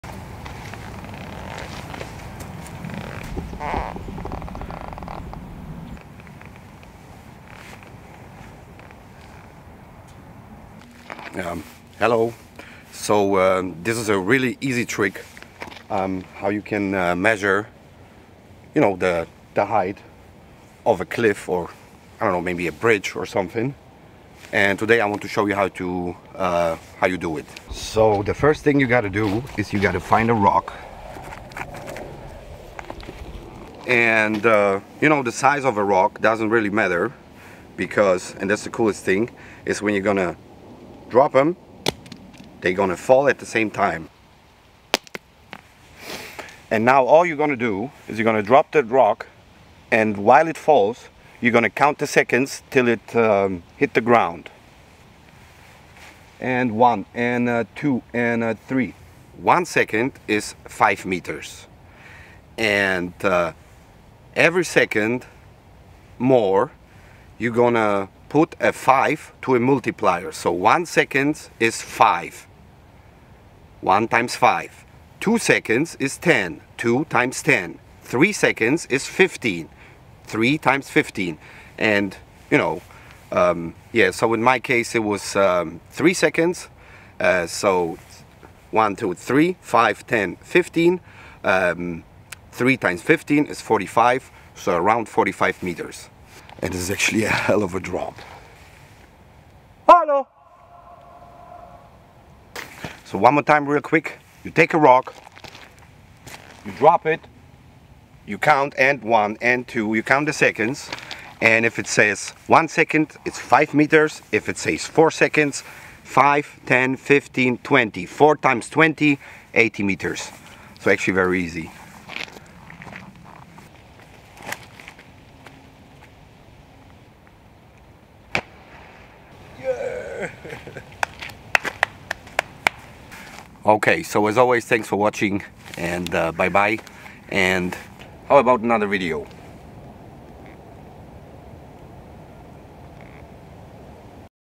Rule of Thumb for Height .... Using a Rock ... VERY EASY
This is a super easy way for measuring the height of what ever you are standing on .